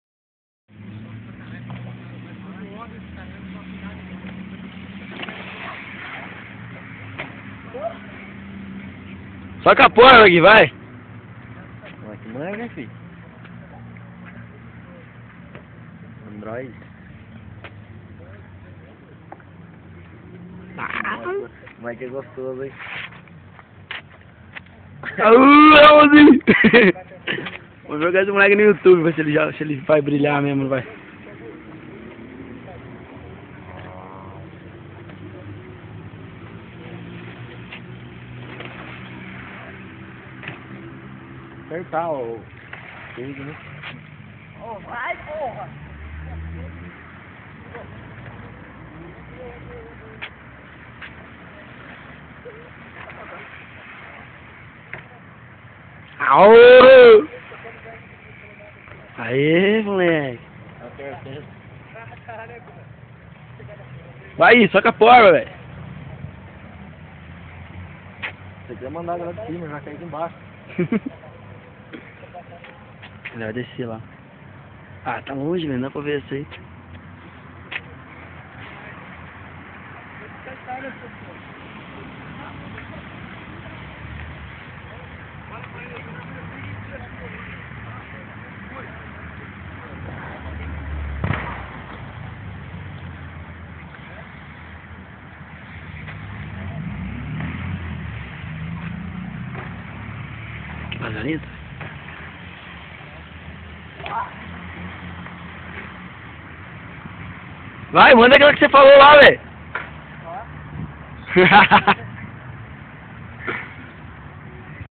Guilherme BMX Serra Negra
Guilherme... andando na pista em SERRA NEGRA SP

mandando vee nas raaampaa. haha

isso ai mlk!